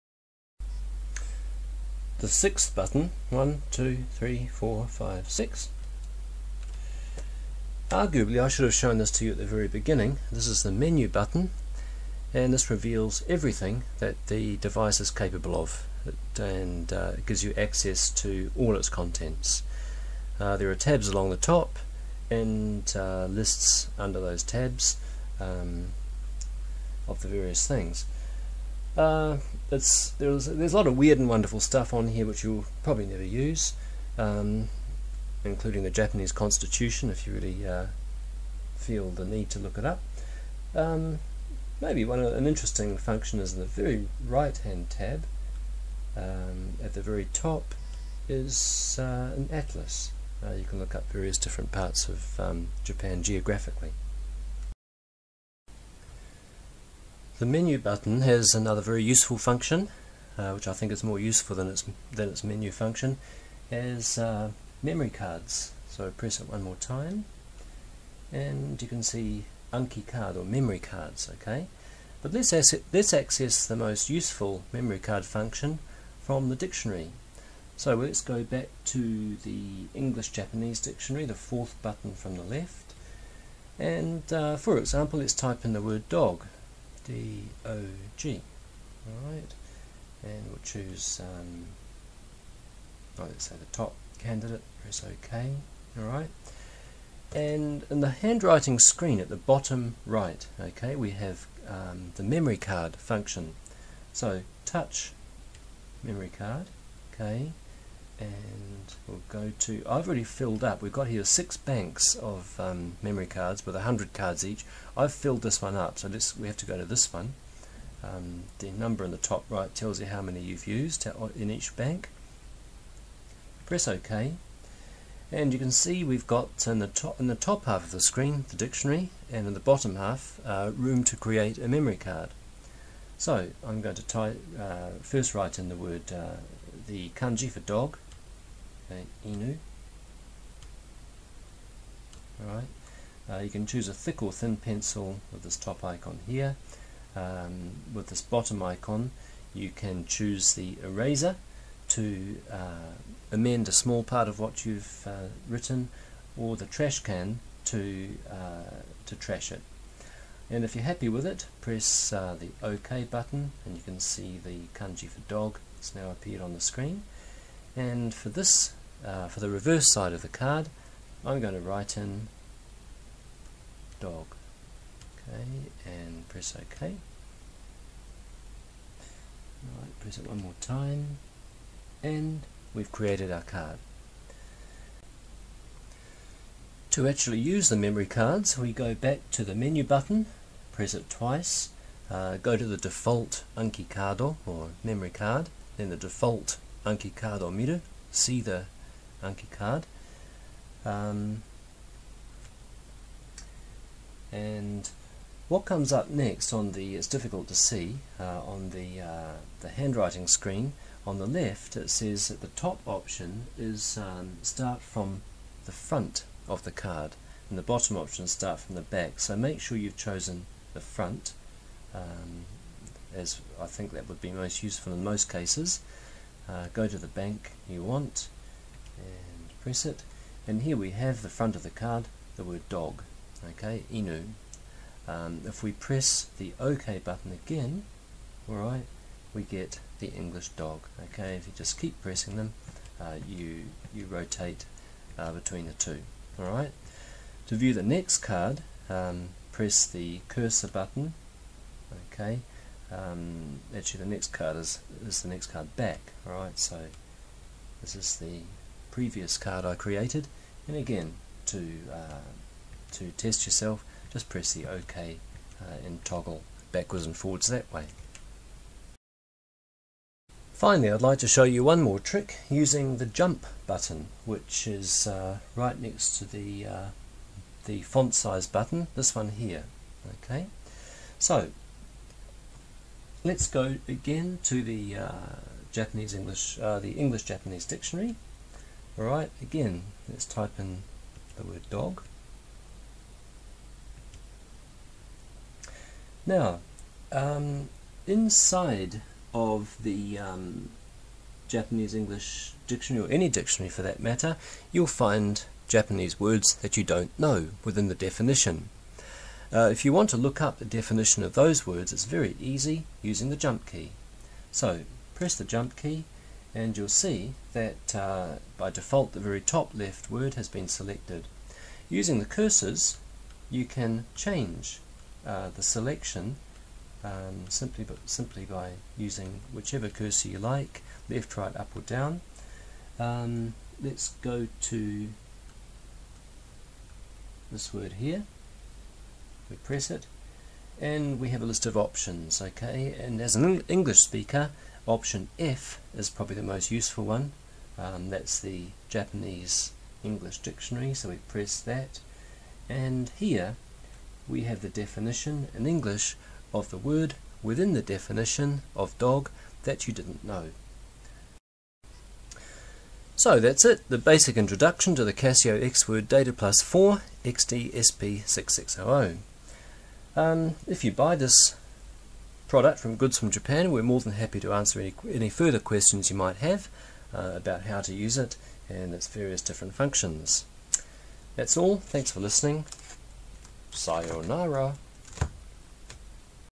CASIO EX-word DATAPLUS 4 - guide 3
The 3rd of 3 easy tutorials for English-speakers about how to use CASIO's EX-word DATAPLUS 4 electronic dictionary. For customers of